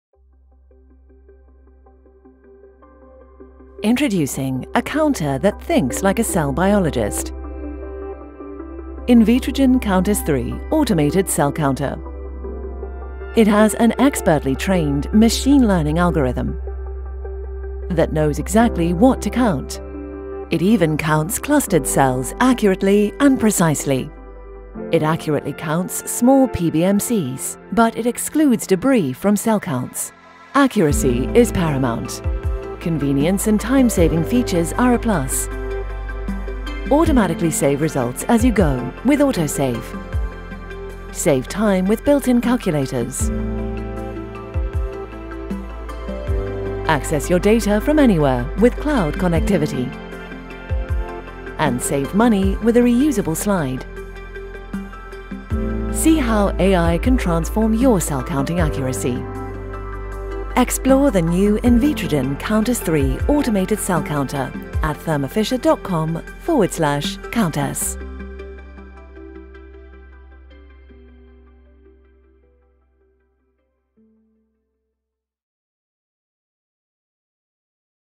Thermo Fisher Countess 3: 3D Animation Video | California Technical Media
Looking for a more efficient and accurate way to count cells? Check out this amazing explainer video on the Thermo Fisher Countess 3! This video showcases how artificial intelligence (AI) is transforming cell counting accuracy with expert machine learning algorithms. With very accurate counting and the convenience of time-saving technology, this device is a must-have for any lab. California Technical Media specializes in creating effective explainer videos for technical products like the Thermo Fisher Countess 3, and can help you showcase your own product's capabilities.